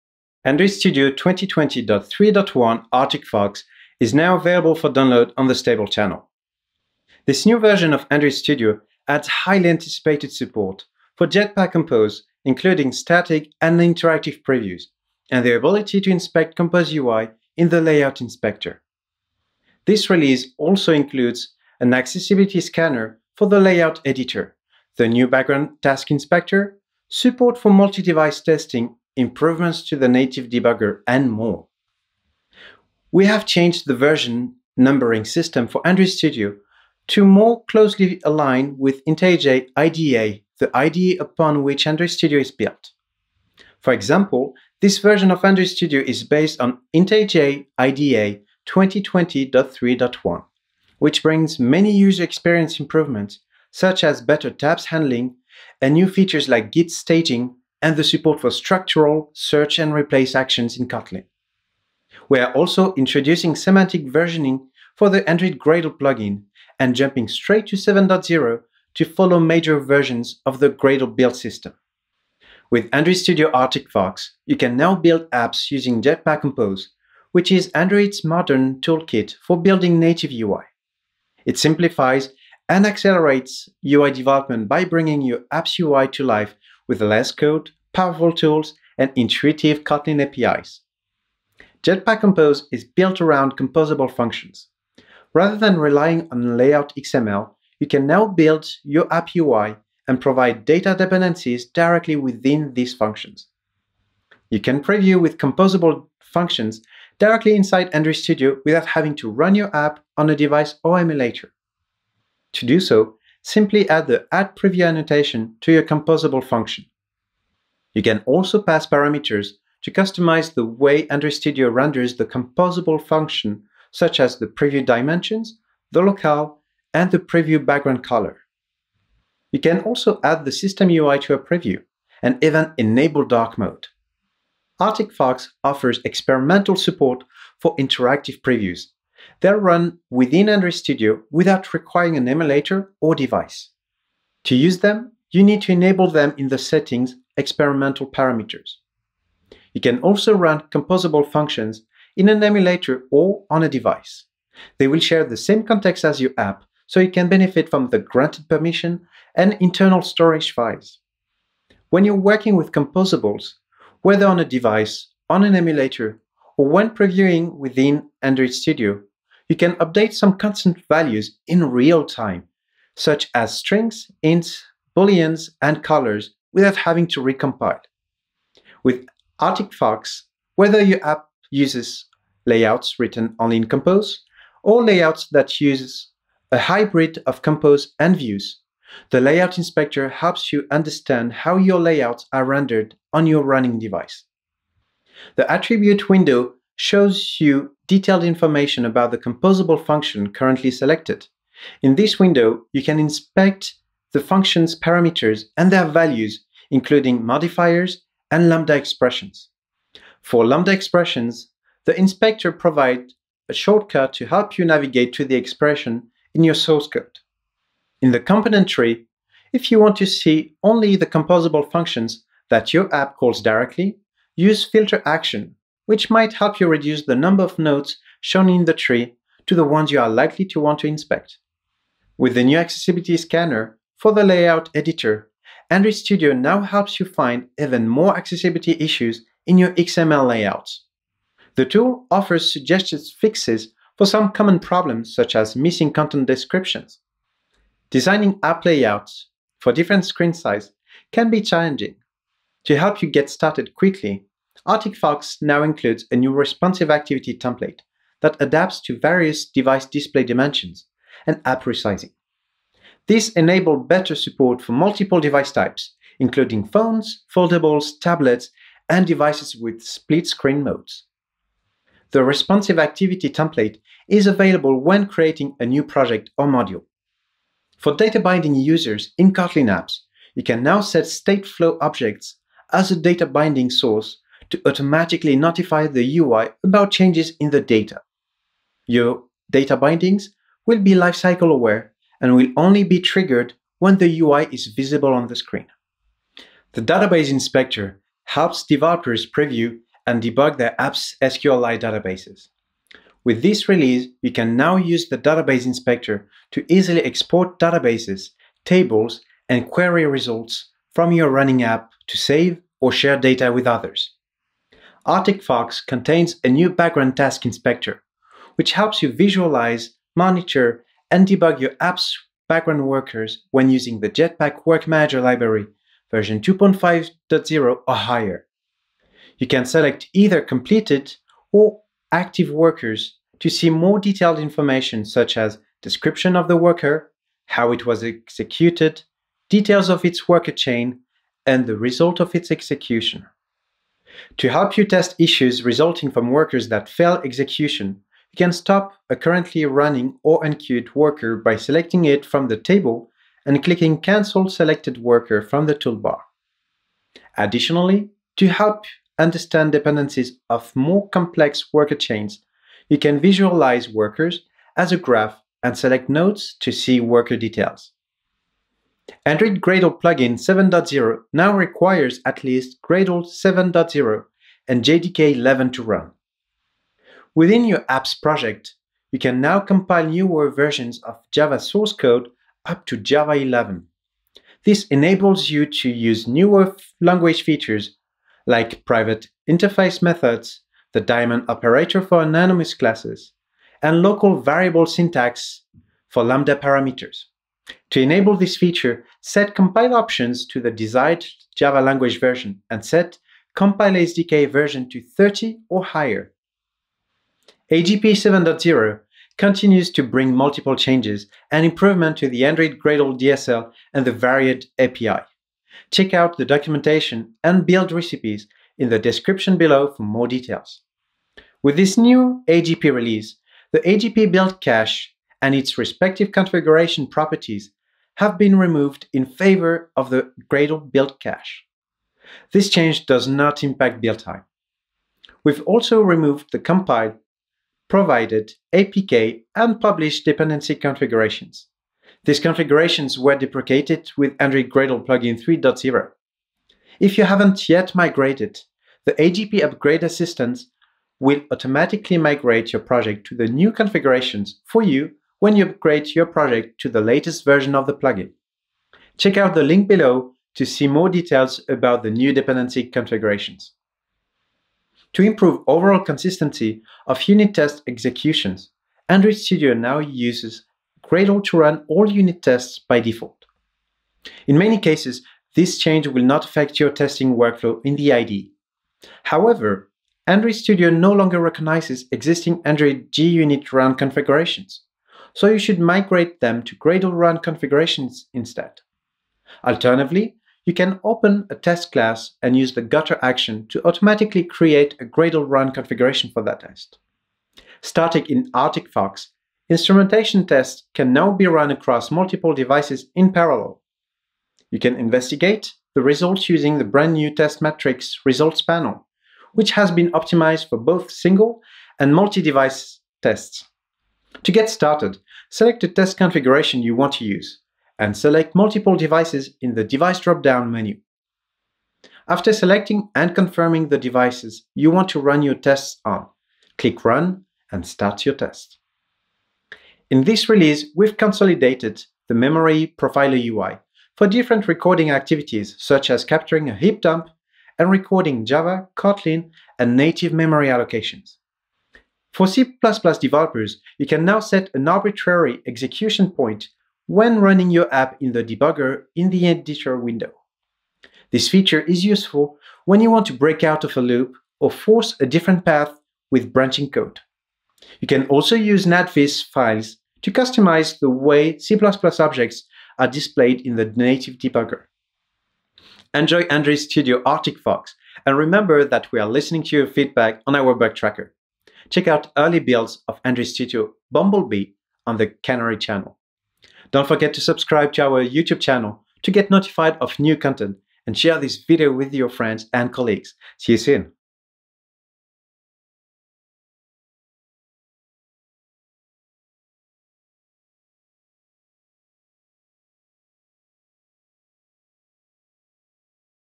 What's new in Android Studio Arctic Fox
Get an overview of what tools and features are available in Android Studio Arctic Fox and the new updates and requirements to Android Gradle Plugin 7.0.

New features:
•Compose support (preview, interactive preview, layout inspector)
•Accessibility Scanner for Layout Editor
•Background Task Inspector
•Test matrix for multi-device testing
•C++ debugger improvements

product: Android - Android Studio; re_ty: Publish;